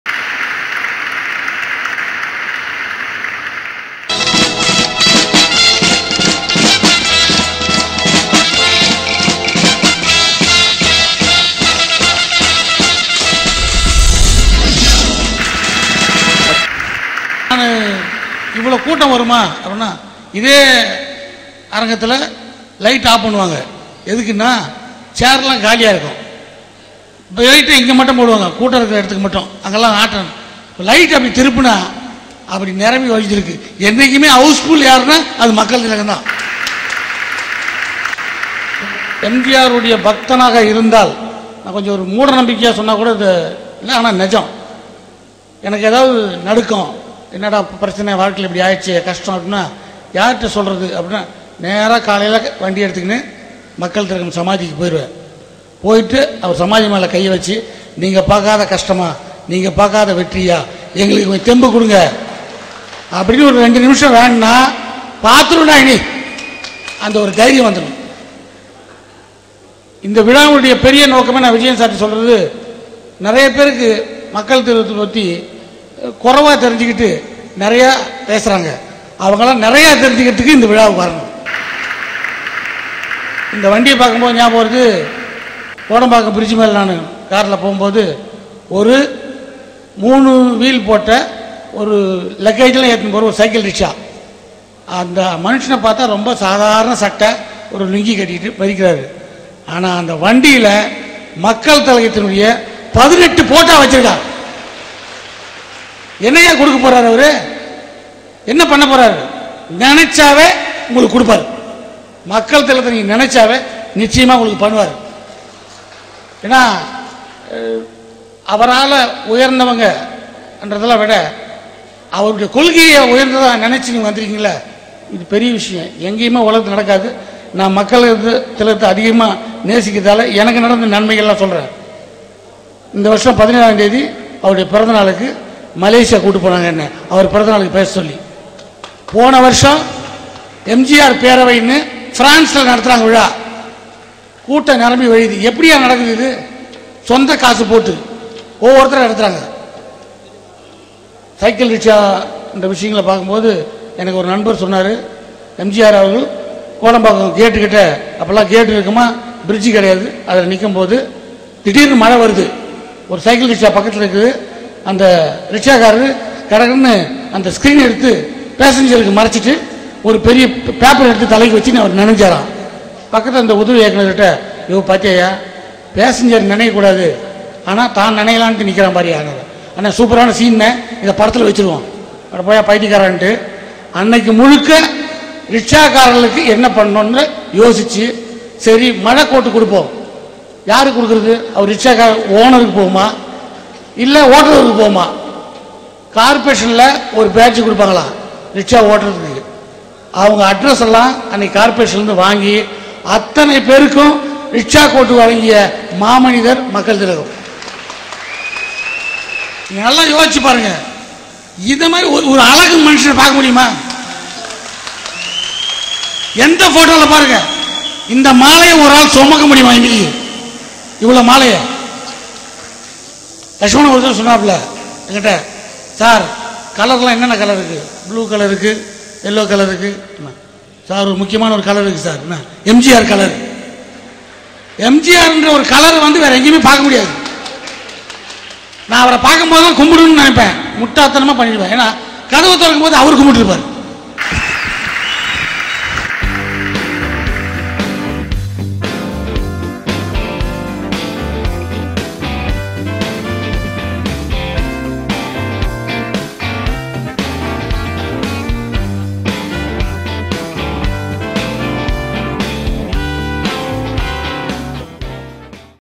Actor Pandiyarajan about MGR | MGR93 | ShowTalks | GoldenMemories
This video was shoot 10 years ago during MGR's 93rd Birthday and we found it after this long time hidden in our server. Happy to share with you all.

Lakshman Sruthi Orchestra - 100% manual orchestra since 1987

got some interesting moments with MGR?
comment it below!!